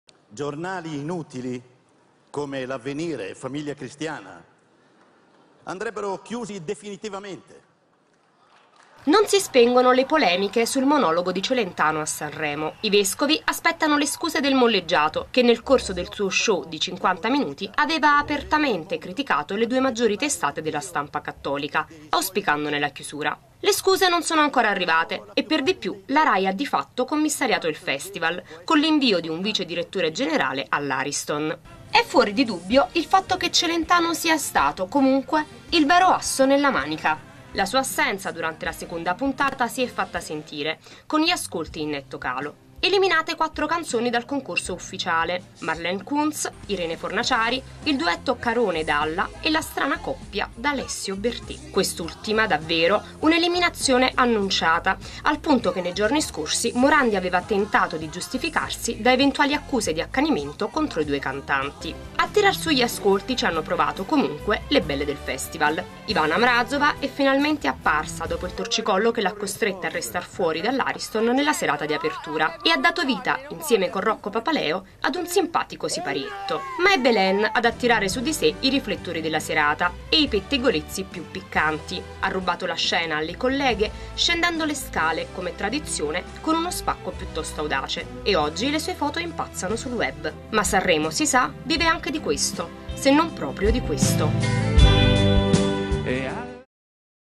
Sanremo, canzoni, vescovi e Belen
La seconda serata del Festival registra polemiche su Celentano e l'esclusione di D'Alessio-Berté. Servizio di Marcella Maresca. Reporter Nuovo, Scuola di Giornalismo Luiss